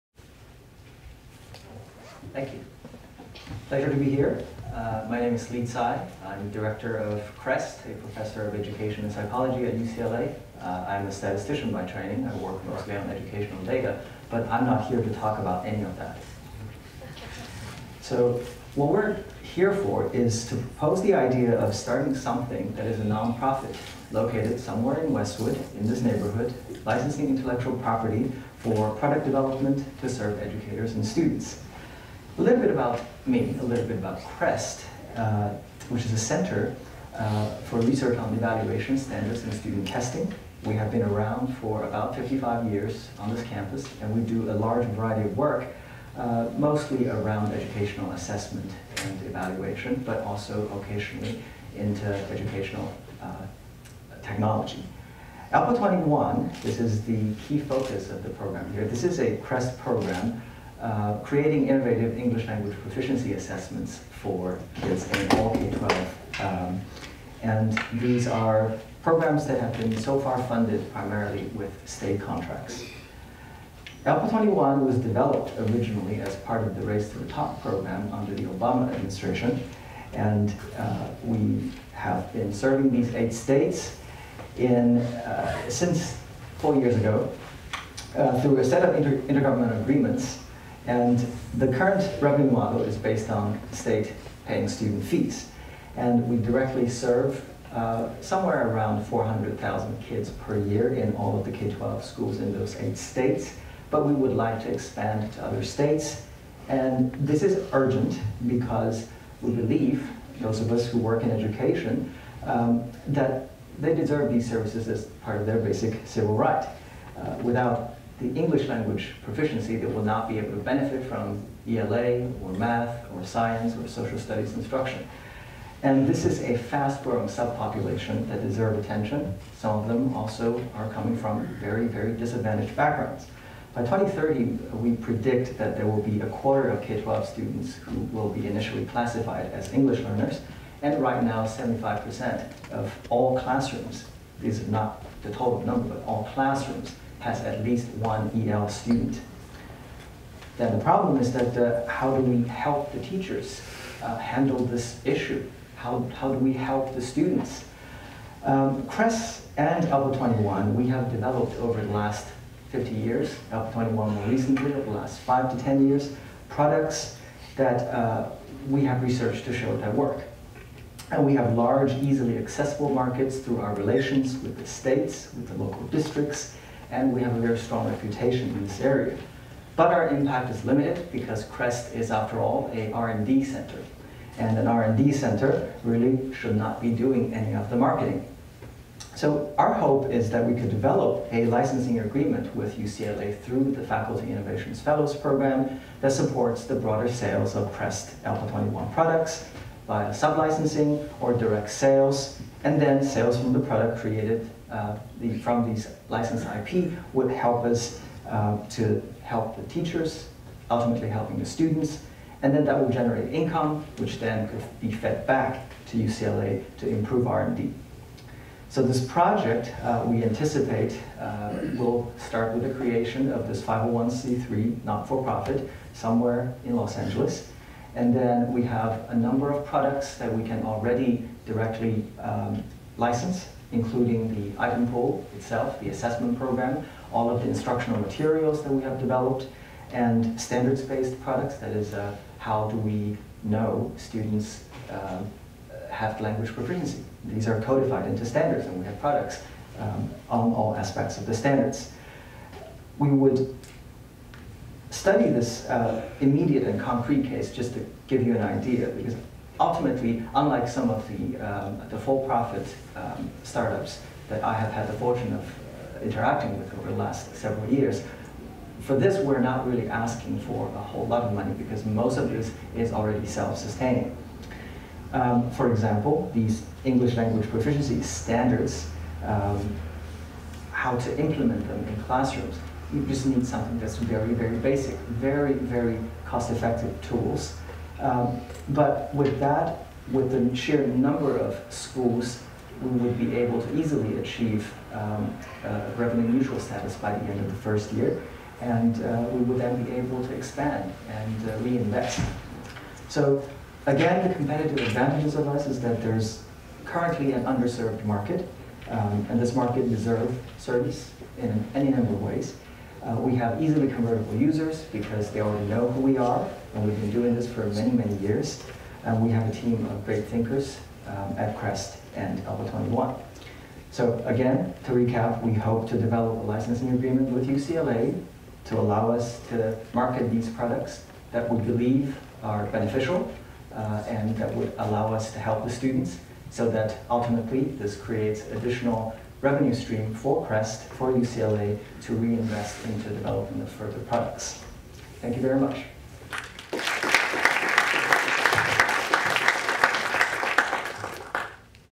LI CAI, BESSTT
Faculty Innovation Fellows Pitch Showcase:  May 22, 2019

UCLA's Faculty Innovation Fellows, a new program designed to support and amplify faculty entrepreneurship at UCLA.

Li Cai, Professor, Education & Psychology: BESSTT *Building Educational Solutions for Students, Teachers, and Trainers (research based educational assessment tools)